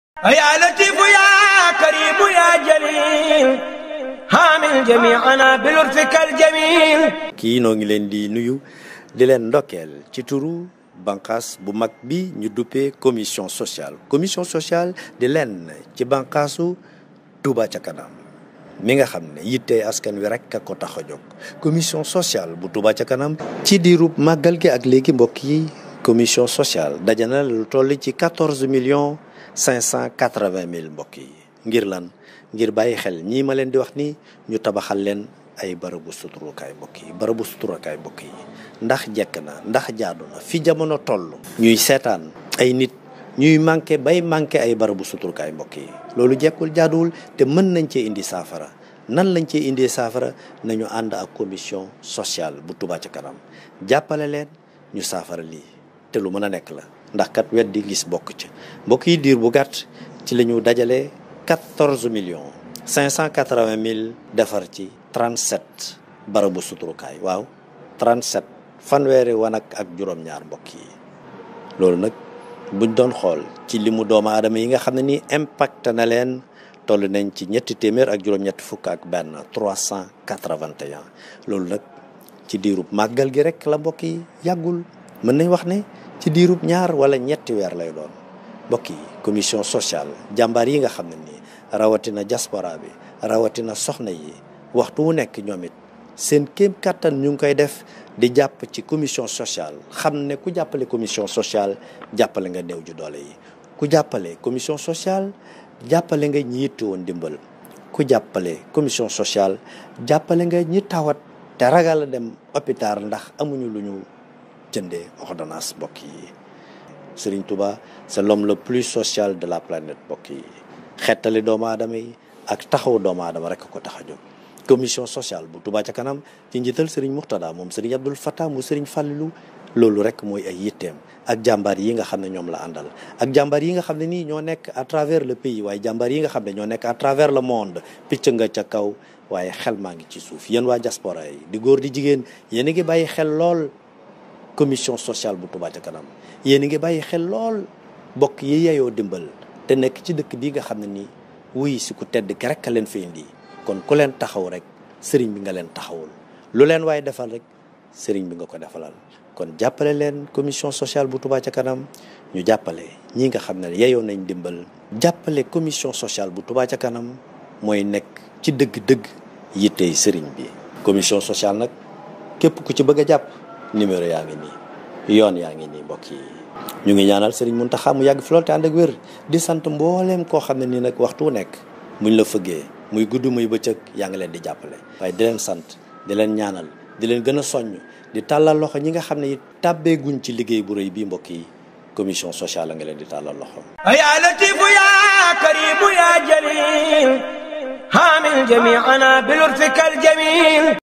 sociale Touba ça kanam: vient de construire 37 toilettes pour des familles démunies à Touba
Touba ça kanam